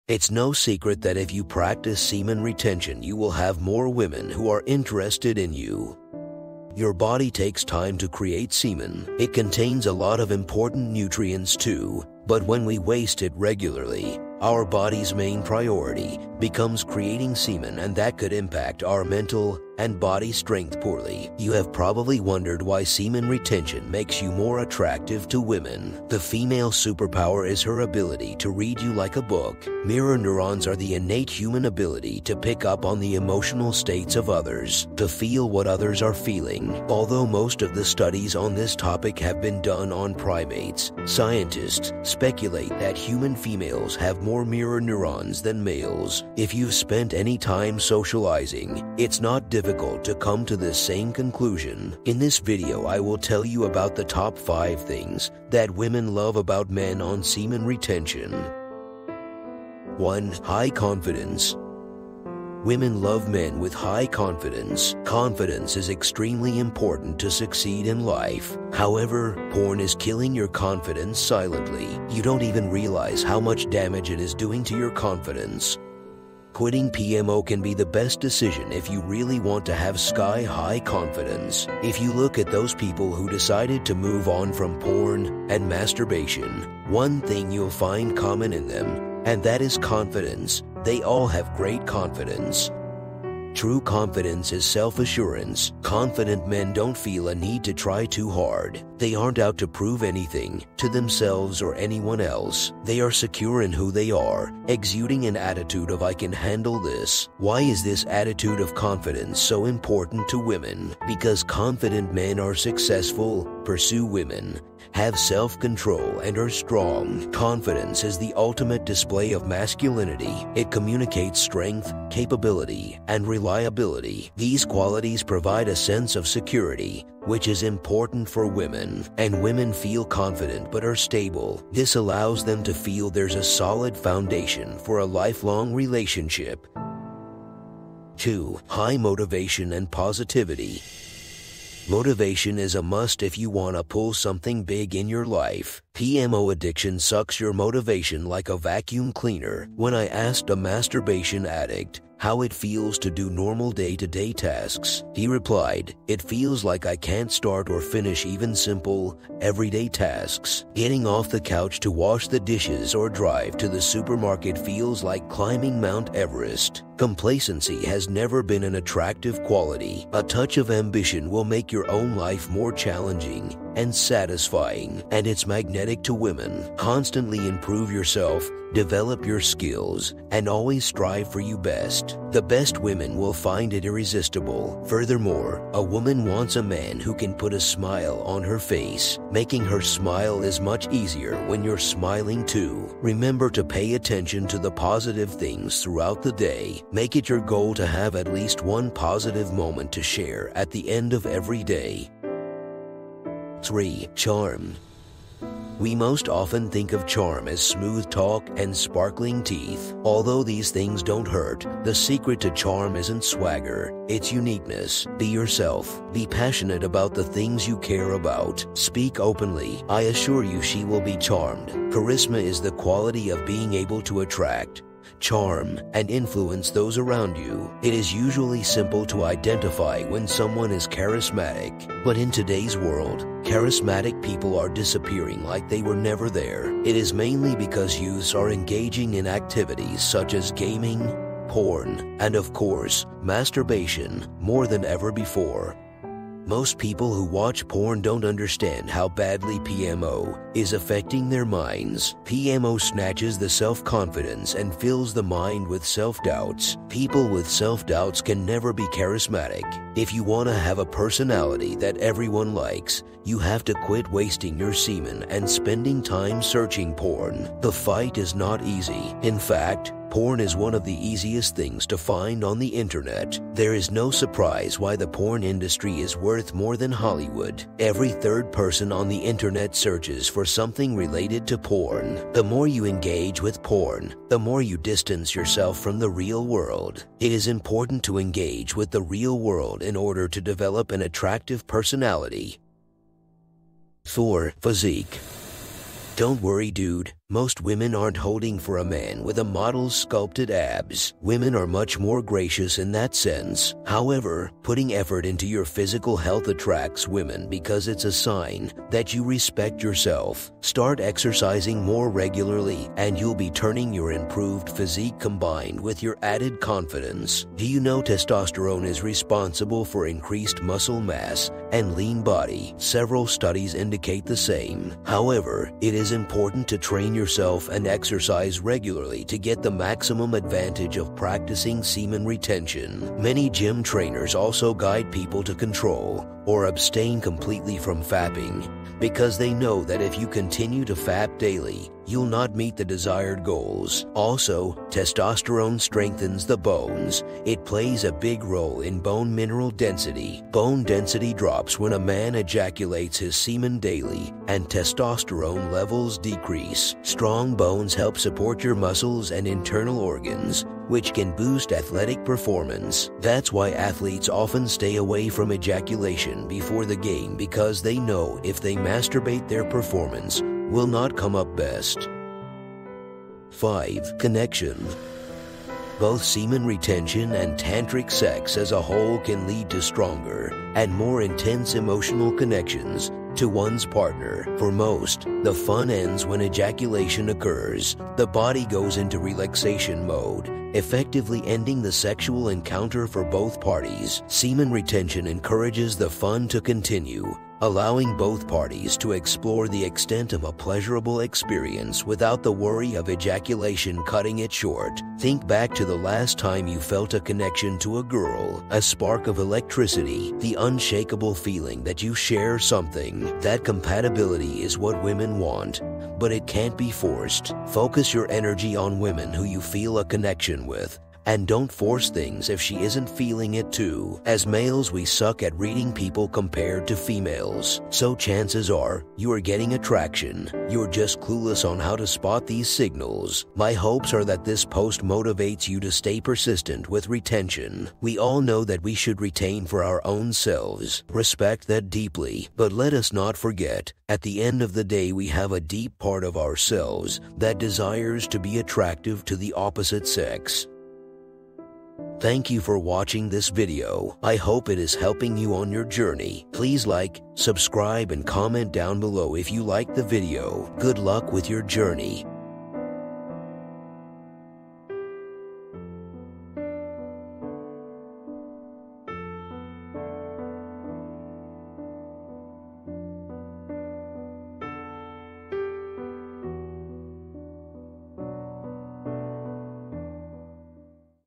Semen Retention Female Attraction: Top 5 Things Women Love About A Man On Semen Retention | NoFap
Semen Retention Female Attraction: Top 5 Things Women Love About A Man On Semen Retention
One of the most common semen retention benefit is the female attraction. Semen retention attraction is an usual benefit on your semen retention story and semen retention journey. In this video i will show you the Top 5 characteristics a women loves at a man on semen retention.Semen Retention Attracts women

It's no secret that if you practice semen retention you will have more women who are interested in you.

Your body takes time to create semen. It contains a lot of important nutrients too, but when we waste it regularly, our body’s main priority becomes creating semen and that could impact our mental and body strength poorly.

You have probably wondered why semen retention makes you more attractive to women.
The Female Superpower is her ability to read you like a book.
Mirror neurons are the innate human ability to pick up on the emotional states of others.
To feel what others are feeling.
Although most of the studies on this topic have been done on primates, scientists speculate that human females have more mirror neurons than males.

If you’ve spent any time socializing, it’s not difficult to come to this same conclusion.

In this video I will tell you about the top 5 things that women love about men on semen retention.

My hopes are that this post motivates you to stay persistent with retention.
We all know that we should retain for our own selves.
Respect that deeply.
But let us not forget, at the end of the day we have a deep part of our selves that desires to be attractive to the opposite sex.
I hope it is helping you on your journey. Good luck with your journey!